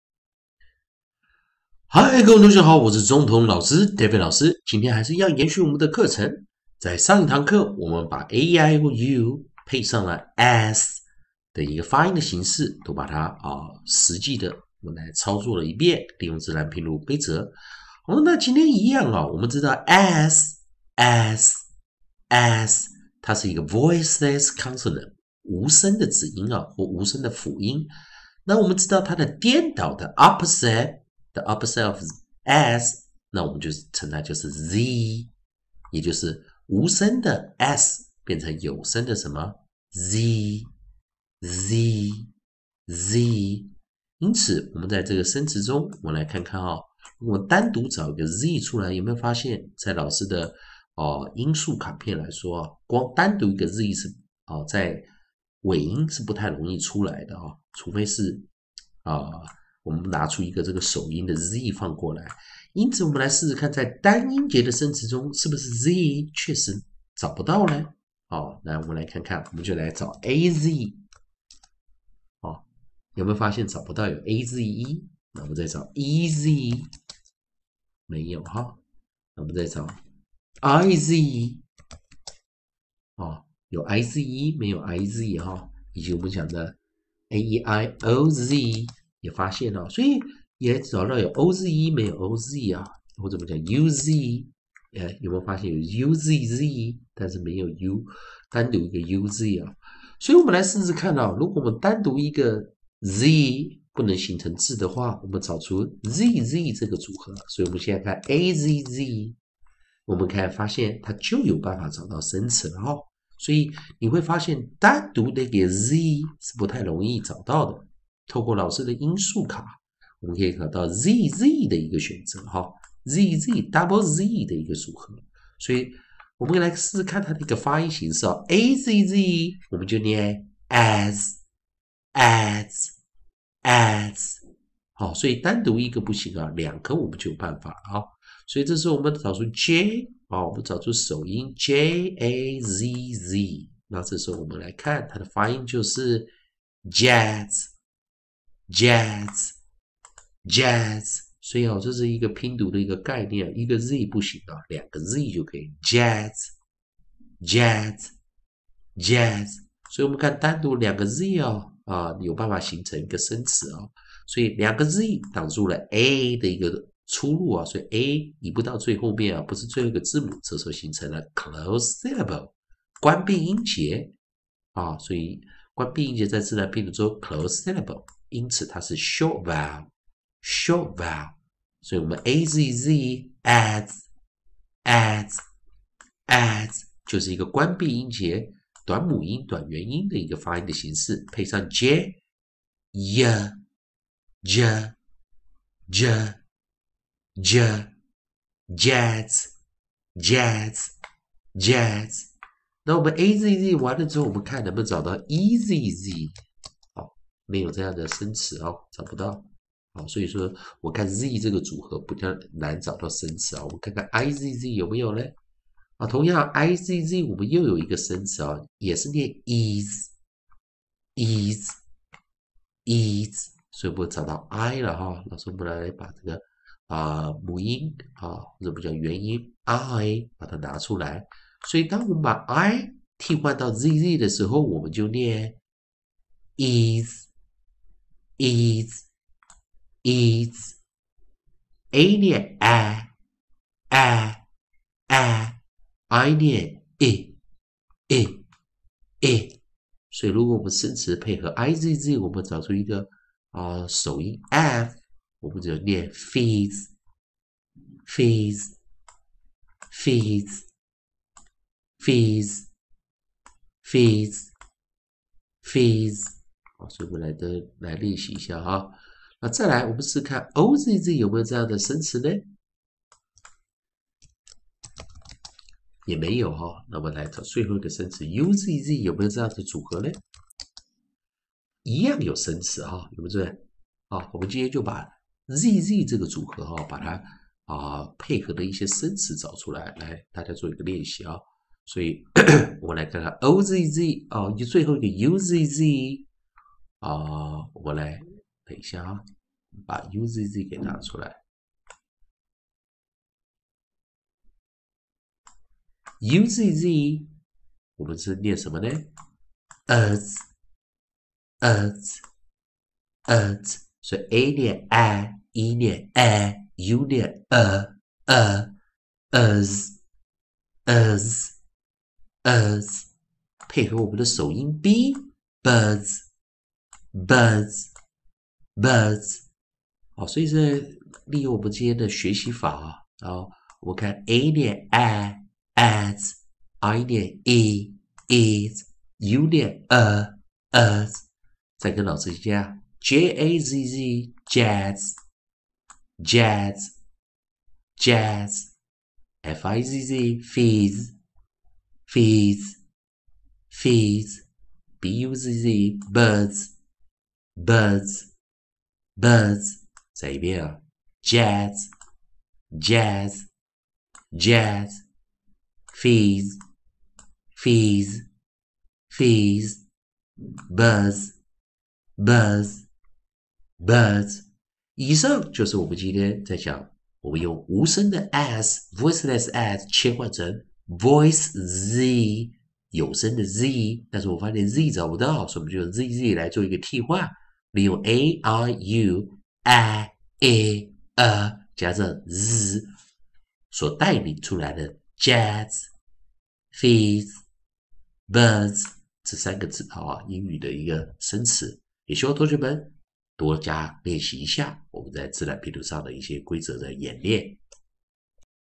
丁宗同美語V7(21) : -azz, -izz, uzz 韻音組合 (自然發音、國際音標、音素)
🔠 韻音/連音 組合【 -azz, izz, uzz 】Rhyme sets 🔠
講解: 丁宗同老師

(VCC Closed) 母子子音 關閉音節 短母音
-azz [æz]
jazz dʒæz
-izz [ɪz]
fizz fɪz
-uzz [ʌz]
buzz bʌz

👥【課程與合作方案 】👥
wechat: davidting88
Line: davidting88